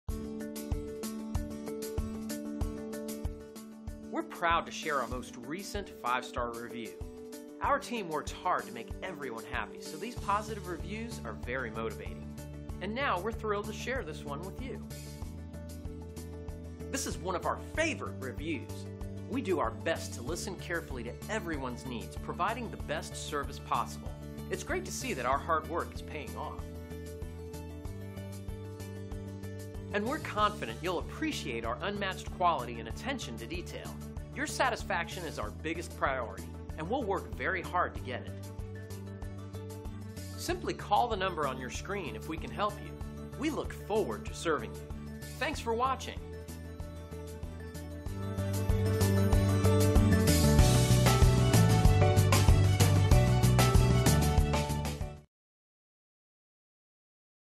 Tampa Bay Electronic Systems Review by Joel B
5 Star Rating

I would highly recommend TBES, they helped recommend an awesome home theater system and home audio system at a reasonable price. The service level was fantastic during the process and they have been very helpful in recent months when I needed tech help after the installation was long complete. We have been enjoying the systems and are very thankful we included them in our home renovation.
by Joel B

Tampa Bay Electronic Systems
Tampa FL